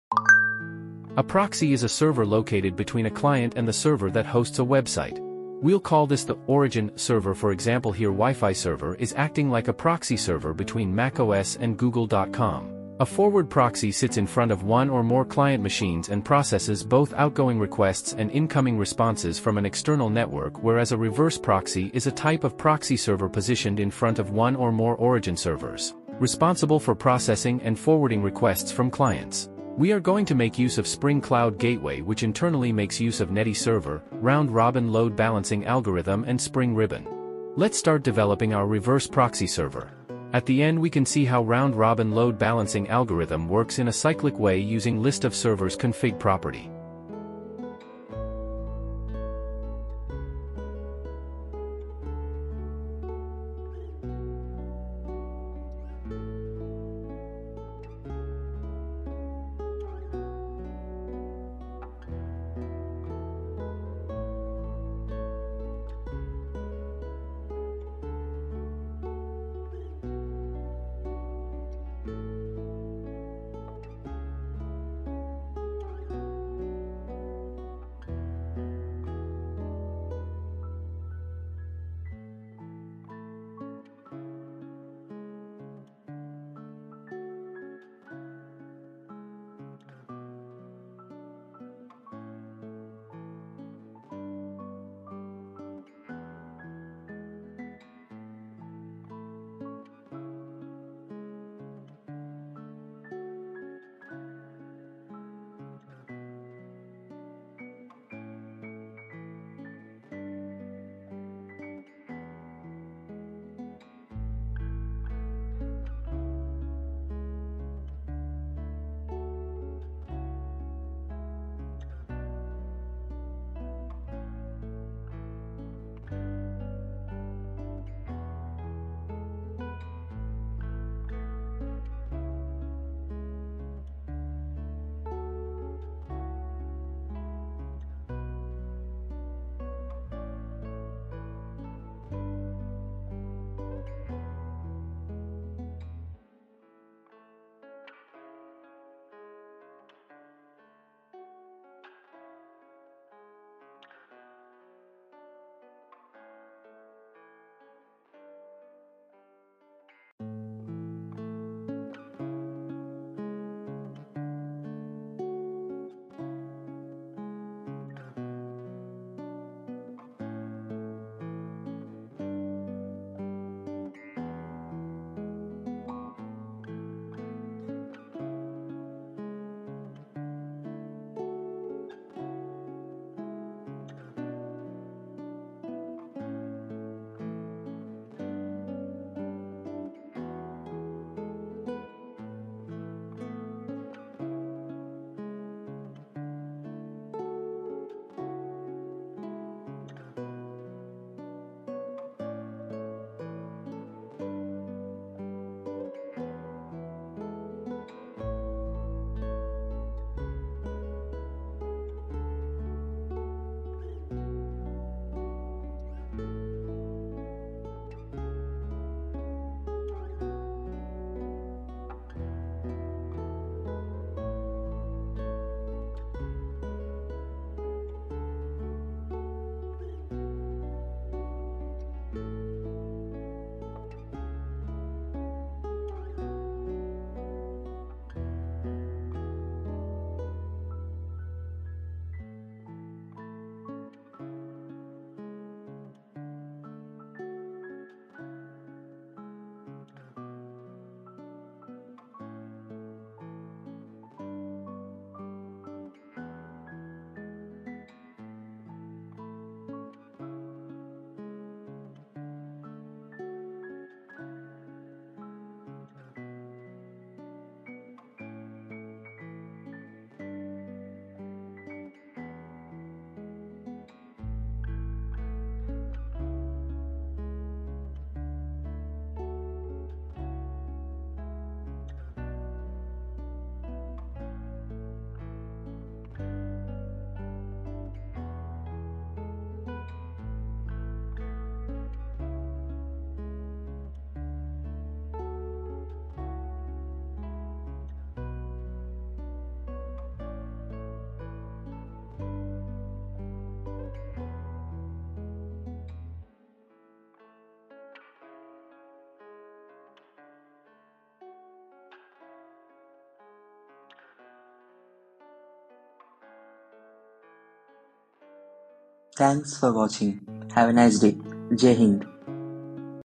Spring Boot: Reverse Proxy Server Implementation
This video can used for the reverse proxy server implementation using spring cloud gateway, netty server, spring ribon and round robbin load balancing algorithm.